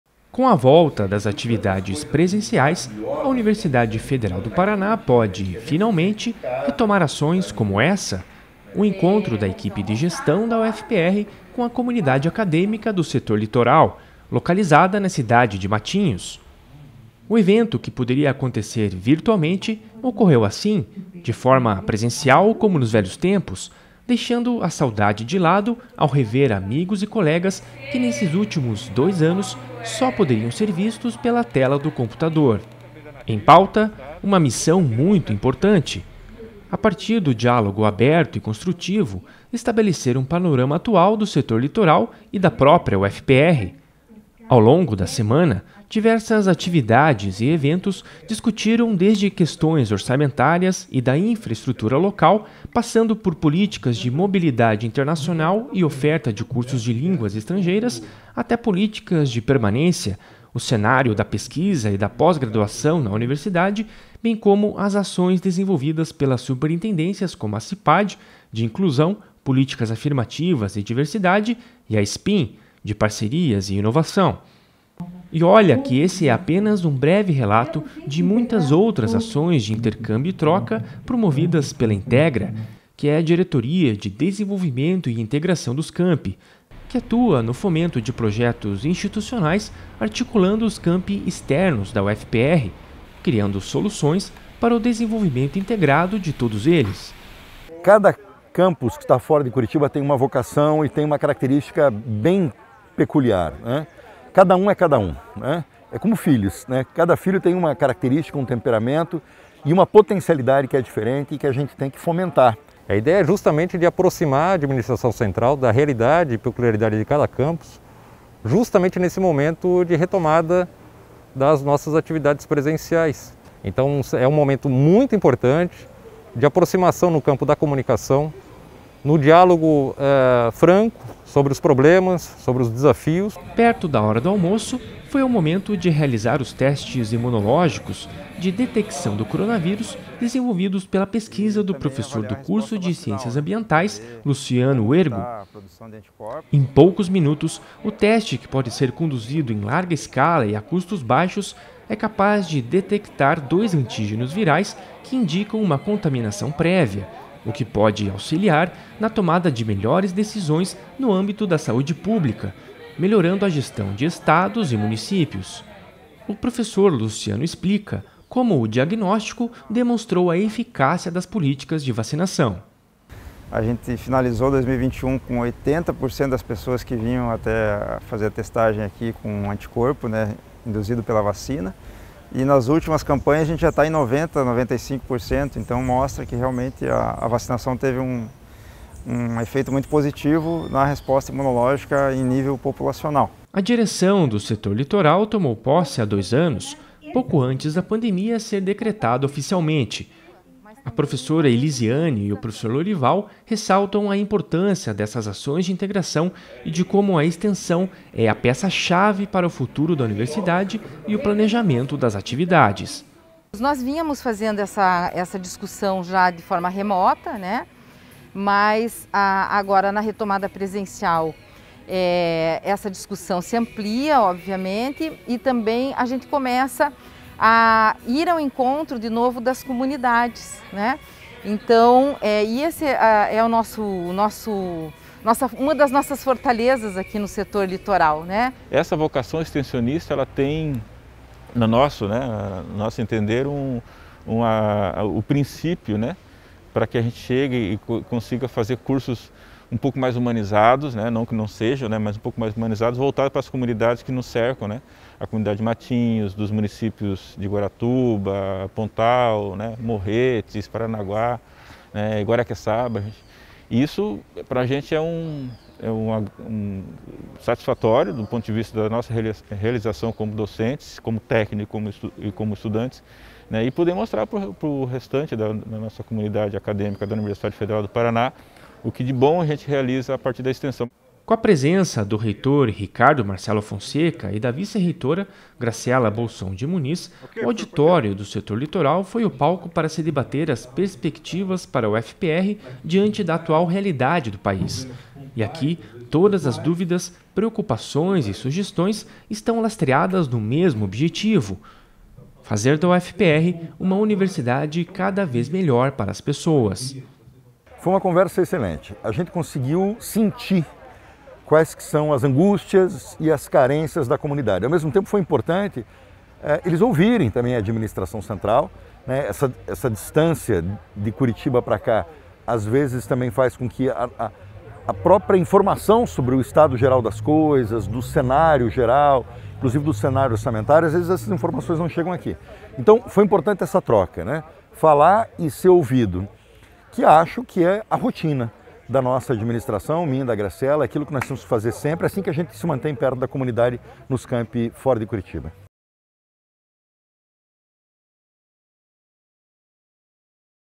Informa | Setor Litoral recebe visita da equipe de gestão da UFPR
Com o retorno das atividades presenciais, a equipe de Gestão da UFPR também retoma ações de encontro com a comunidade acadêmica. Nesta semana, representantes da Reitoria estiveram no Setor Litoral, em Matinhos, para participar de diversas atividades e eventos.

Além do reencontro presencial e das trocas de experiências, a visita também serviu para debater assuntos como questões orçamentárias, infraestrutura, políticas de permanência, pesquisas e pós-graduação, entre outros.